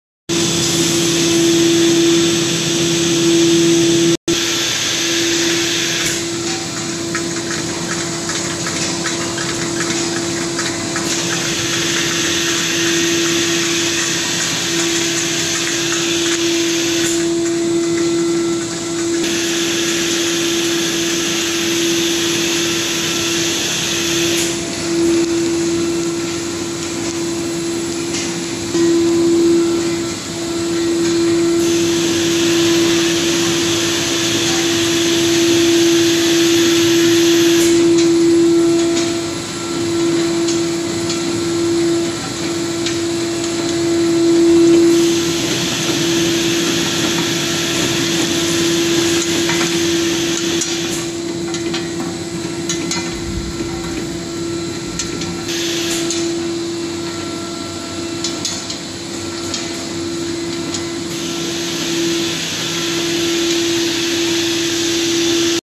Dishwasher tablet packing machine with water soluble film|PVA film wrapping machine
This pillow packing machine is packing up the two layer dishwasher tablets with water soluble film.